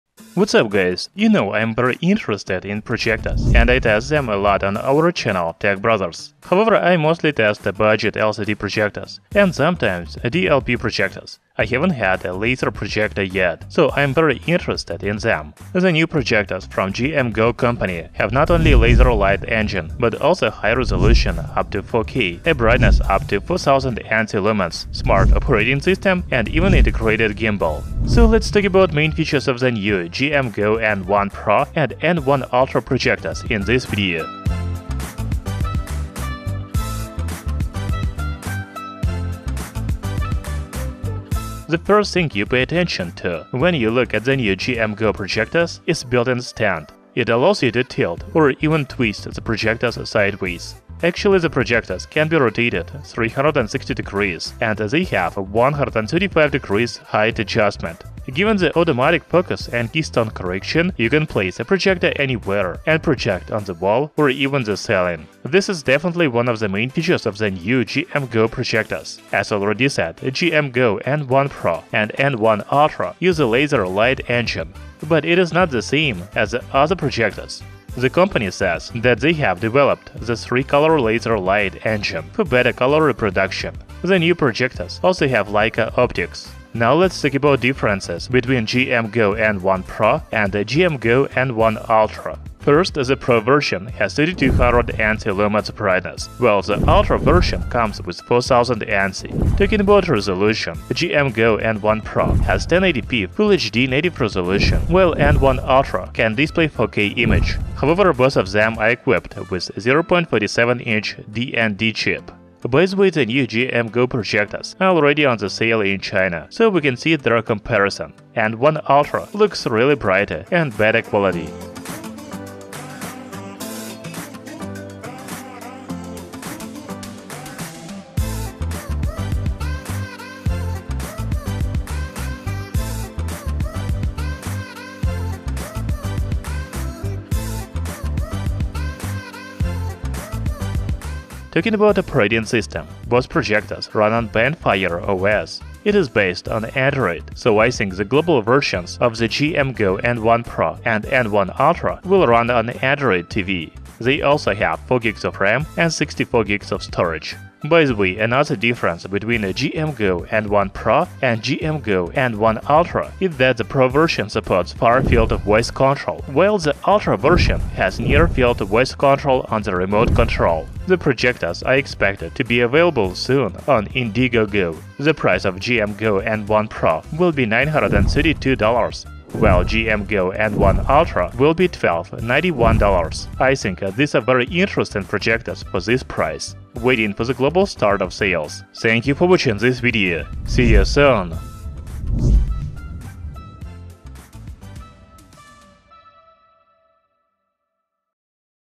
JMGO N1 Pro and JMGO N1 Ultra: Laser Projectors with 360° Gimbals!
JMGO N1 Pro and JMGO N1 Ultra are the new laser projectors with 1080P and 4K native resolution, 3200ANSI and 4000ANSI Lumens brightness, Bonfire OS, and other features.

JMGO N1 Pro Features:
Lumens: 3200 ANSI lumens
Standard Resolution: FHD (1920*1080)
Display Chip: 0.47''DMD
Display Technology: DLP
CPU: MT9669
RAM: 4GB
Storage: 64GB

JMGO N1 Ultra Features:
Lumens: 4,000 ANSI lumens
Standard Resolution: 4K (3840x2160)
Display Chip: 0.47''DMD
Display Technology: DLP
CPU: MT9669
RAM: 4GB
Storage: 64GB

HashTags for searching